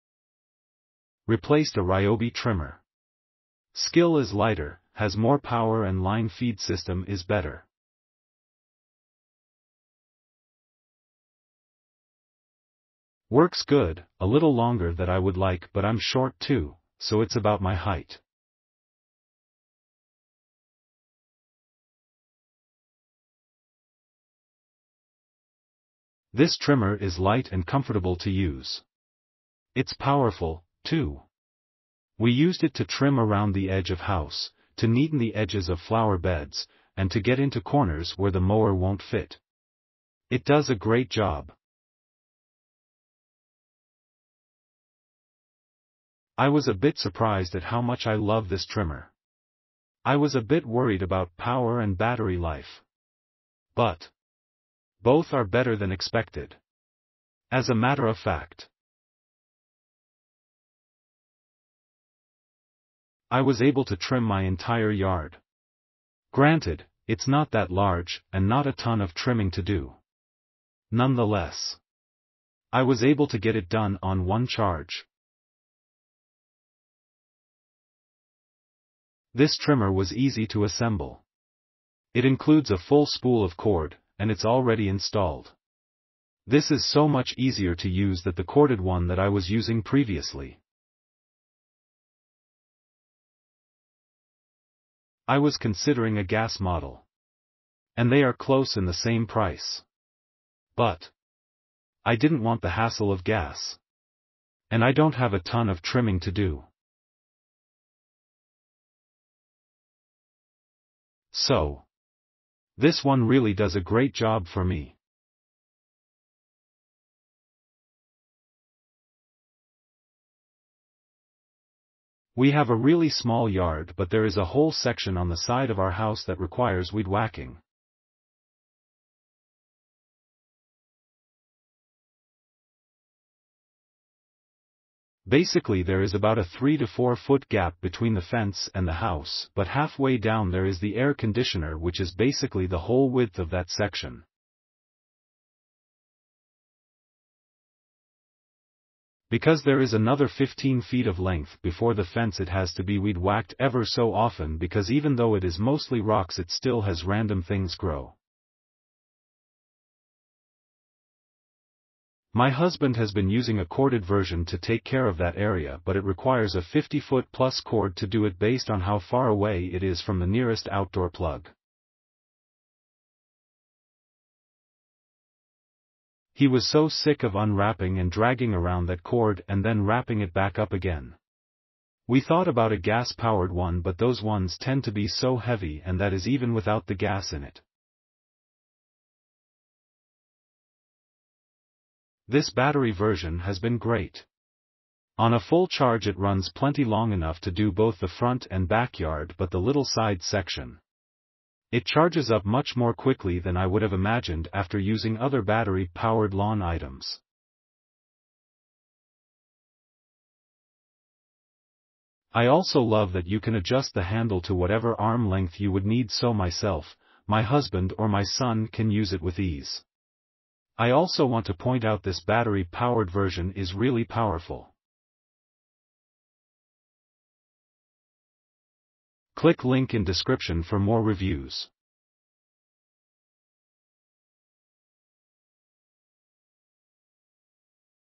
Top 5 | 40 14-Inch Brushless 40V String Trimmer Kit Includes 2.5Ah Battery and Auto PWR Jump Charge
-SKIL LT4818-10 PWR CORE 40 14-Inch Brushless 40V String Trimmer Kit Includes 2.5Ah Battery and Auto PWR Jump Charger
Replaced a Ryobi trimmer. Skil is lighter, has more power and line feed system is better.
works good, a little longer that I would like but I'm short too, so its about my height.
This trimmer is light and comfortable to use. It's powerful, too. We used it to trim around the edge of house, to neaten the edges of flower beds, and to get into corners where the mower won't fit. It does a great job.
I was a bit surprised at how much I love this trimmer. I was a bit worried about power and battery life.